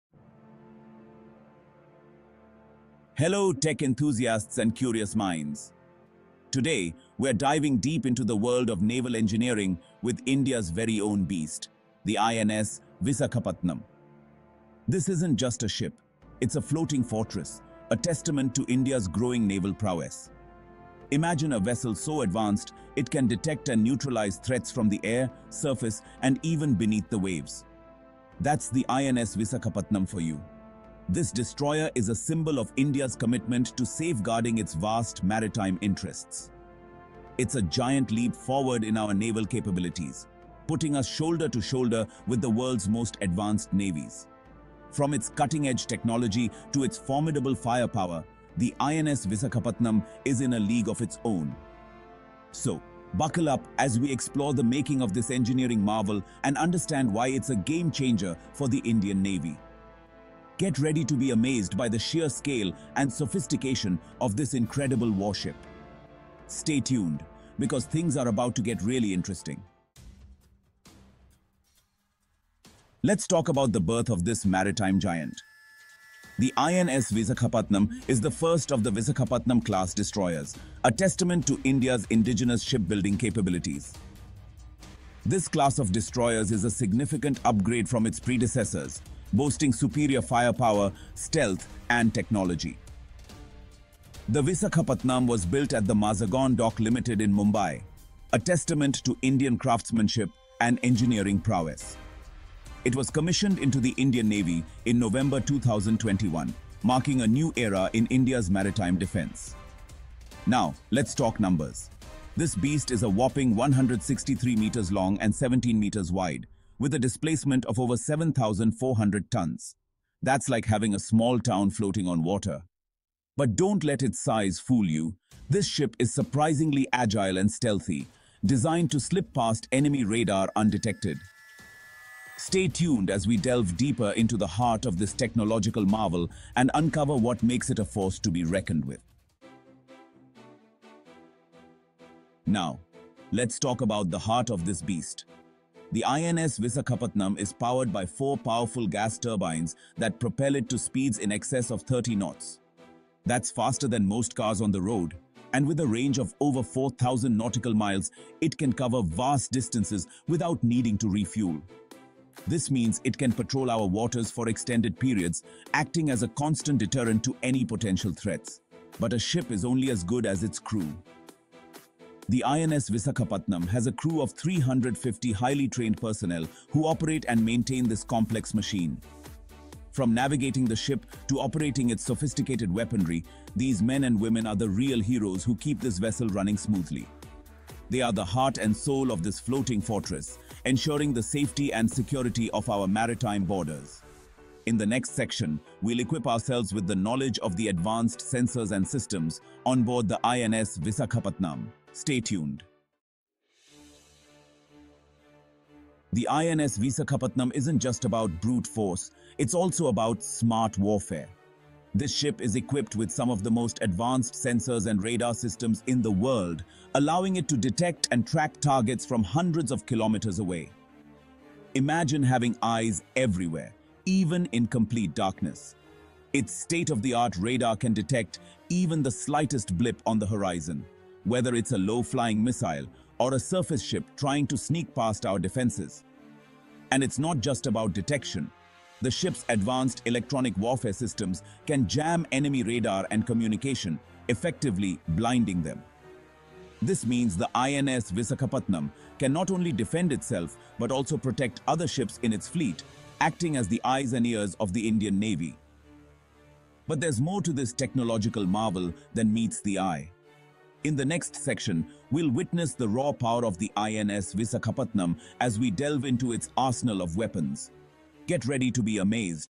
🇮🇳 INS Visakhapatnam: India's DEADLIEST Destroyer 🚢 | Defense in Short India
Dive deep with Defense in Short India as we explore the INS Visakhapatnam, India's most advanced and powerful destroyer! 🇮🇳🚢  This engineering marvel is packed with cutting-edge technology and weaponry, making it a true force to be reckoned with.  From its BrahMos missiles to its stealth capabilities, we'll uncover everything that makes this warship a game-changer for the Indian Navy. Don't miss out! 🔥